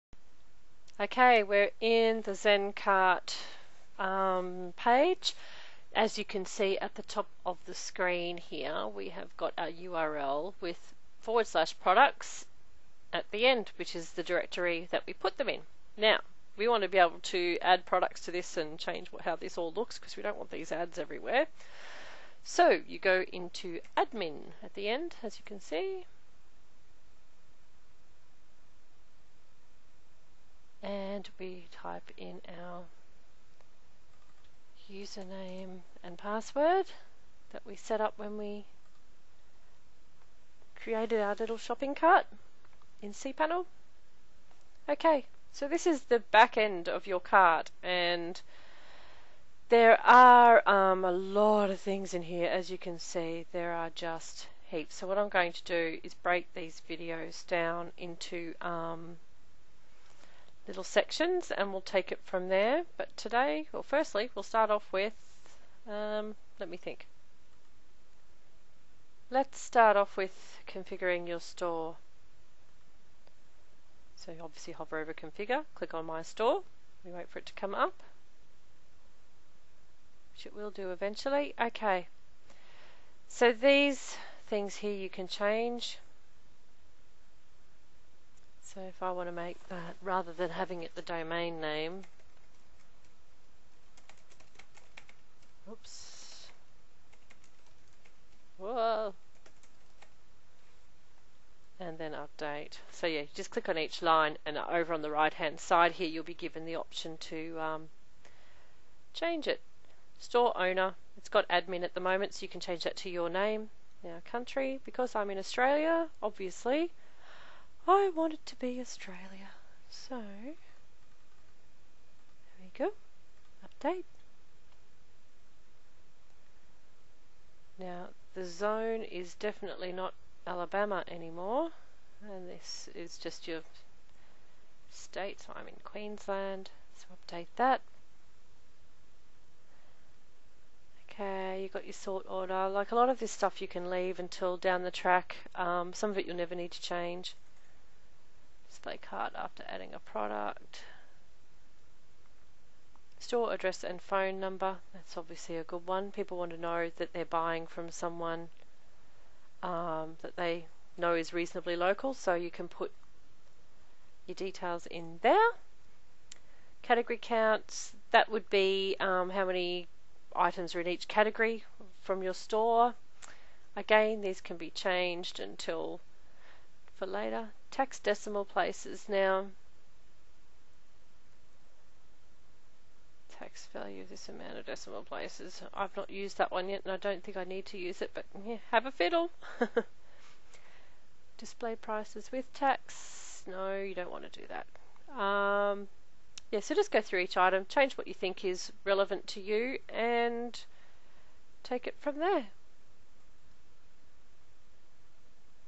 How to configure your Zen Cart store
shows you how to set up your Zen Cart store so you can begin selling online!